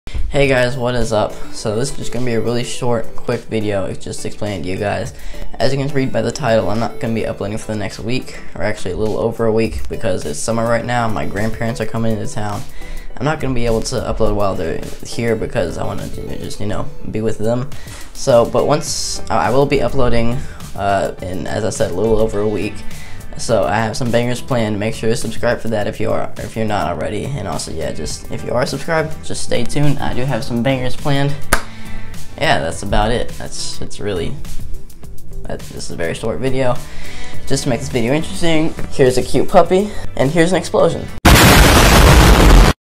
I will not be able to upload for the next week. 😢 Short vid
I can’t upload for the next week